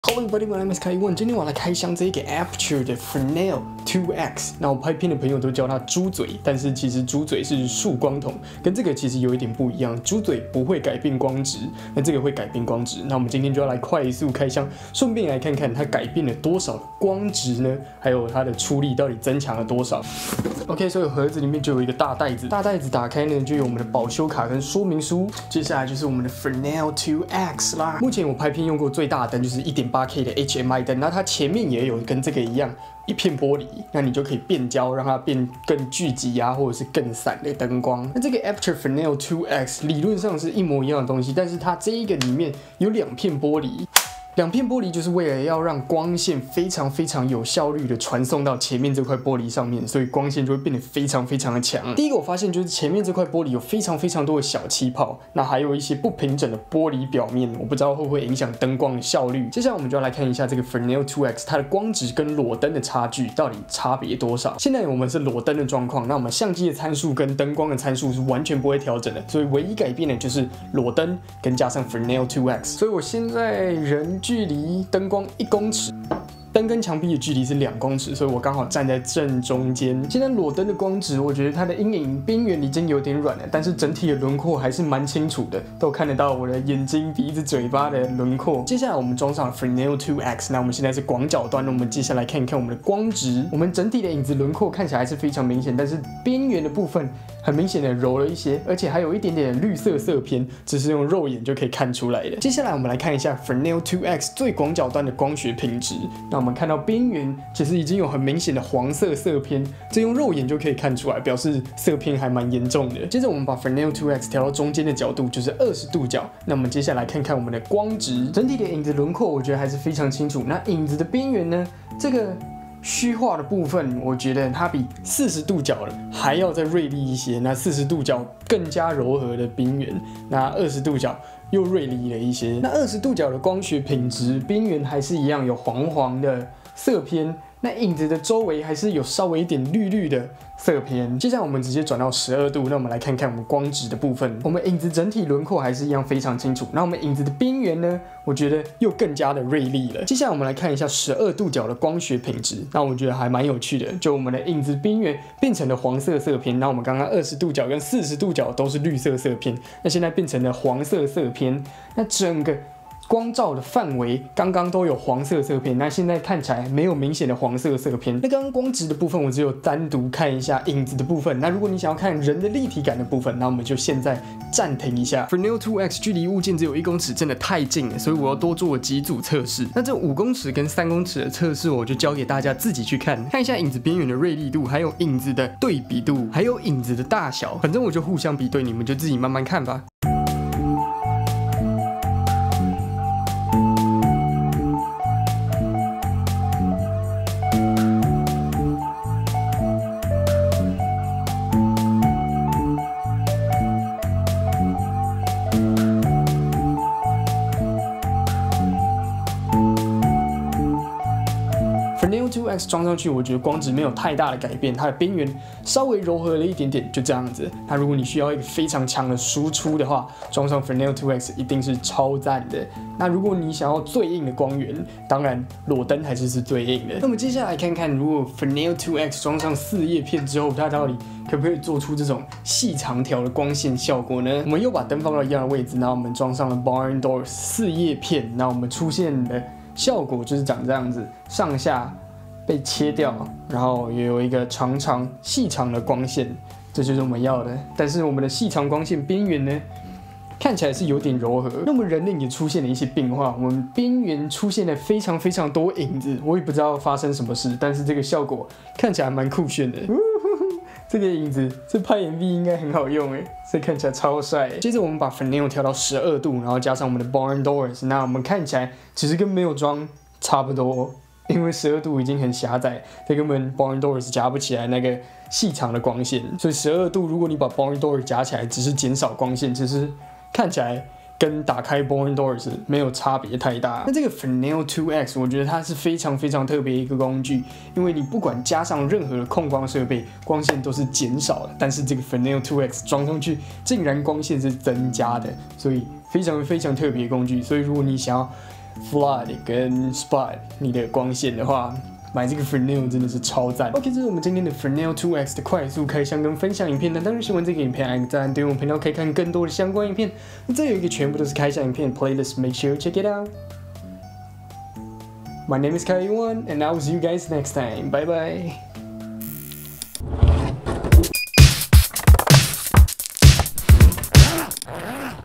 實測 Aputure 愛圖仕 Fresnel 2x 菲涅爾透鏡 買起來 開箱 光質測試 5米 3米 1米 光學品質 光質改變
光質測試 5米 3米 1米 光學品質 光質改變
實測 Aputure 愛圖仕 Fresnel 2x 菲涅爾透鏡 買起來 開箱